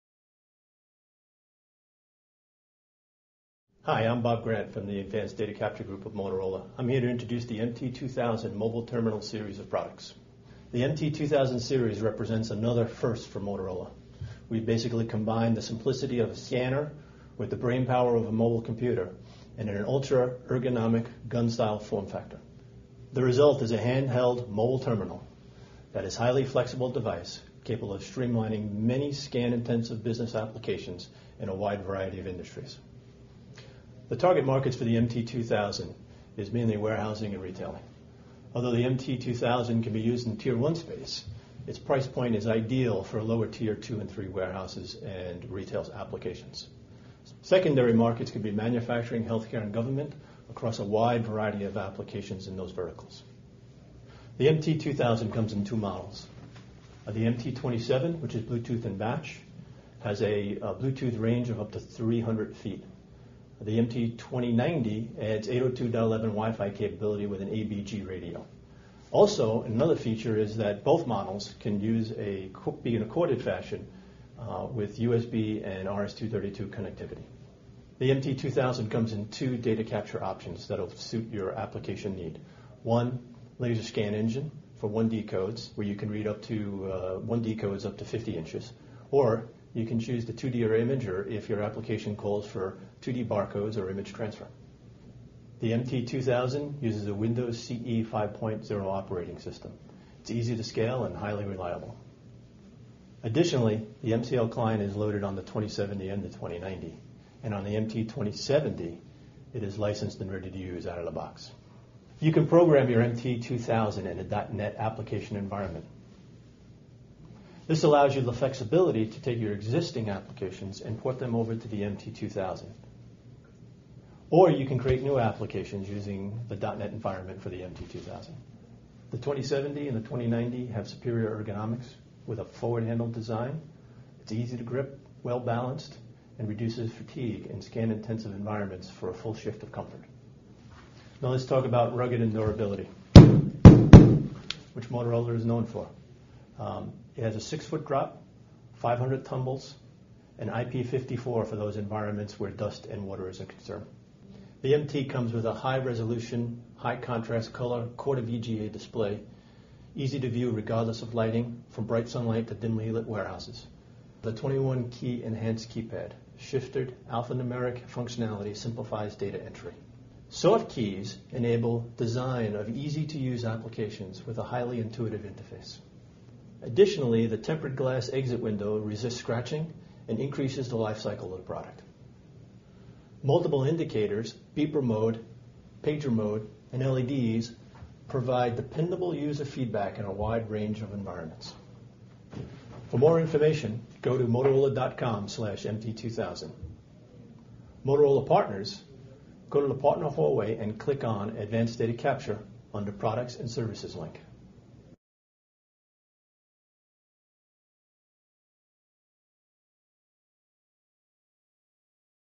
MT2000 Series Handheld Mobile Terminals
Combining the simplicity of a scanner with the intelligence of a mobile computer, these handheld mobile terminals provide advanced 1D/2D bar code, DPM and image capture, as well as the ability to key in and view data.